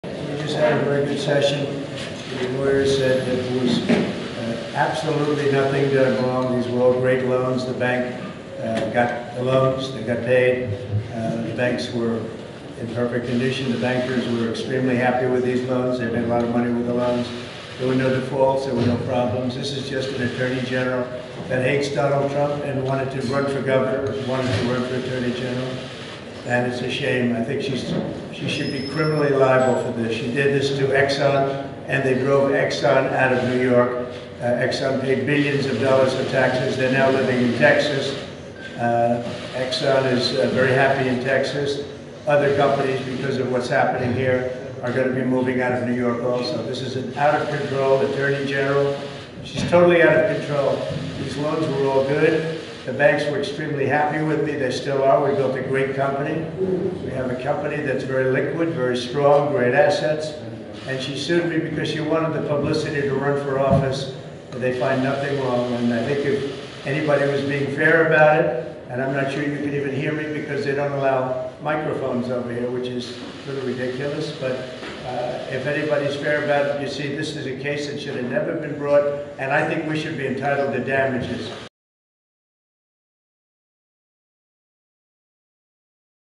Trump Blasts 'Out of Control' NY AG Letitia James; Says He Should be 'Entitled to Damages'
Former President Donald Trump once again ripped into New York Attorney General Letitia James as closing arguments commenced during a civil fraud trial against the former President in Manhattan.
The 2024 Republican Primary frontrunner said outside a courtroom continued to deny any wrongdoing, accusing James of using his prosecution to gain publicity and she should be 'criminally liable,' telling reporters he should be 'entitled to damages.'